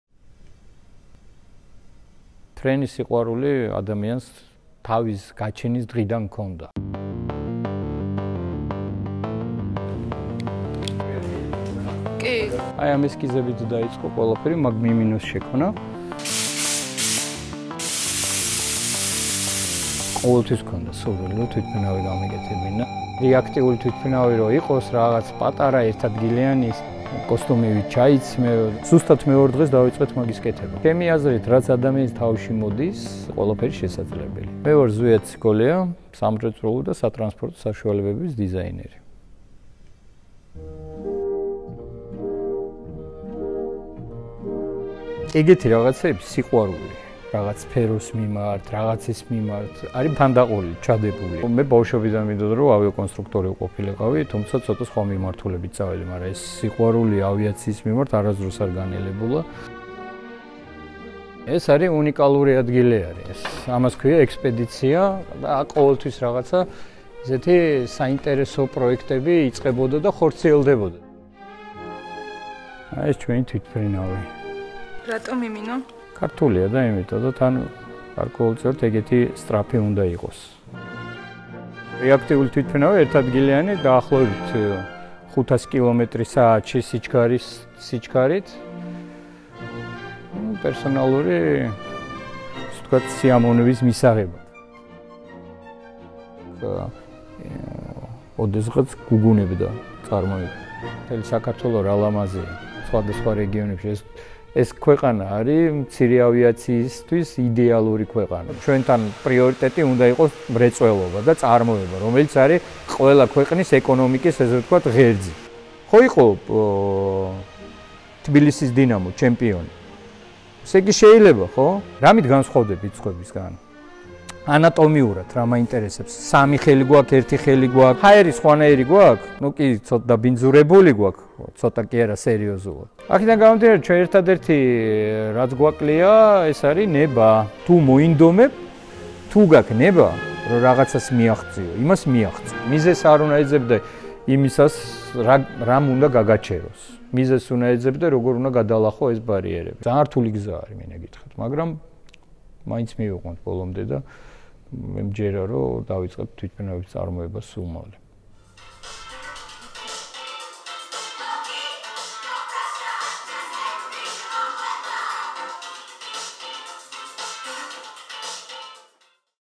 TSIKOLIA | HOLAFILM
როგორ ავაშენოთ თვითმფრინავი? სამრეწველო დიზაინერი ზვიად ციკოლია: "რაც ადამიანის თავში მოდის - ყველაფერი შესაძლებელია".

How do you build an air-plane? Industrial designer Zviad Tsikolia: "Everything you think of - is possible".

FILM N 11
JUNE 2018

საავტოროუფლებები | მარიკა ეჯიბია

HOLAFILM არის ექსპერიმენტალური ვიდეო სიუჟეტების კოლექცია | HOLAFILM is a collection of experimental video stories